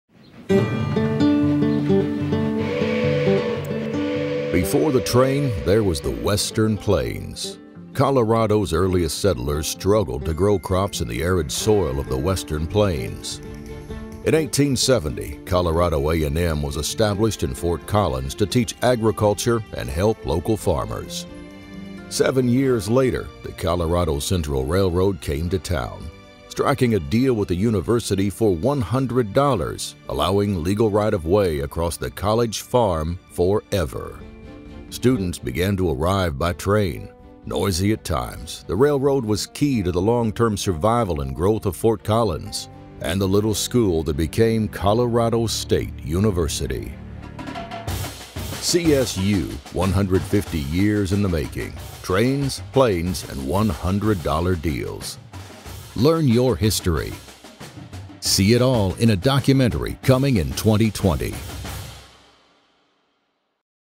Trains, Plains and $100 Deals
Documentary film producer Frank Boring and audiovisual presentation specialist Bryan Rayburn are going through Colorado State University’s film archives to compile a documentary about the University’s first 150 years. Here’s an early glimpse of what they have found. For more stories, an interactive photo slider and a quiz on CSU lore, visit csu150.colostate.edu